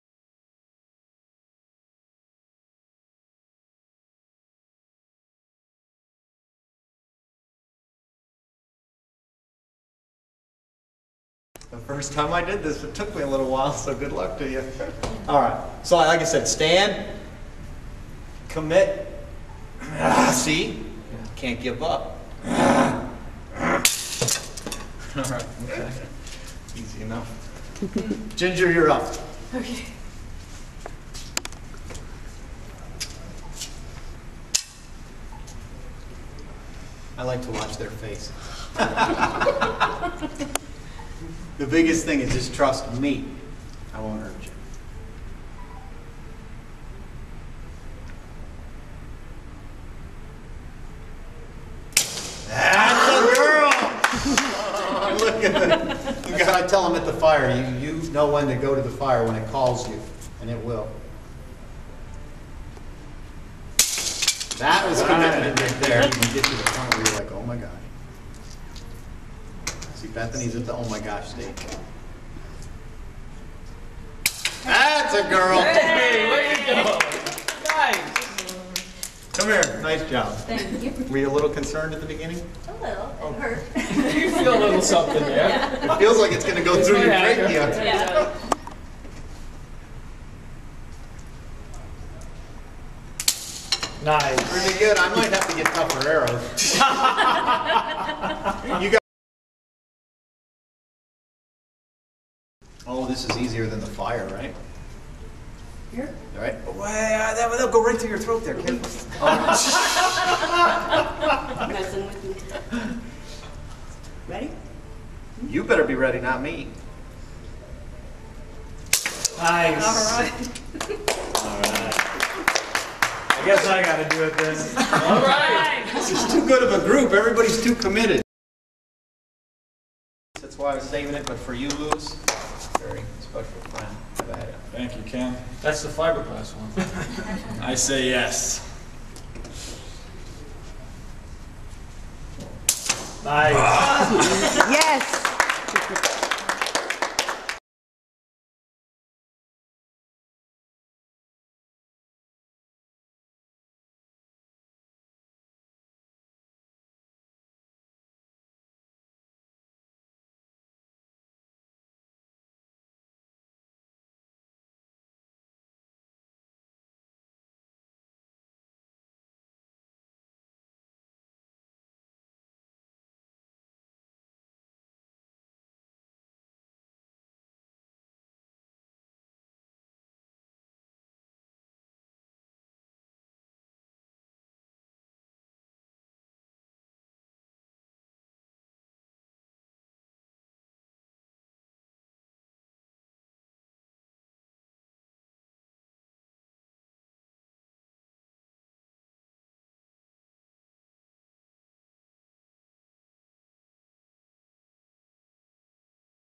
From Fear to Freedom: The Incredible Arrow-Breaking Moment You Need to See!
Experience the transformative power of overcoming fear with Kytka Hilmar-Jezek as she breaks through barriers in a daring arrow-breaking demonstration. Witness firsthand as fear is turned into fuel, igniting passion and propelling individuals to break free from limitations.

At The Yes Movie premiere, witness the fearless display of arrow-breaking, a practice involving placing a wooden hunting arrow against the throat and channeling focus to snap it in half. Human Element's Kenneth Emery wowed the audience with this feat, followed by Kytka's fearless participation, showcasing the triumph over fear and self-imposed limitations.

Join the experts as they demonstrate unwavering courage and push through fears for a breakthrough moment. Arrow-breaking serves as a profound opportunity to challenge limiting beliefs, fostering powerful emotional states that empower individuals to achieve extraordinary results.

Don't miss out on the chance to confront fears head-on and unleash your full potential. Take action now and embark on a journey of limitless possibilities!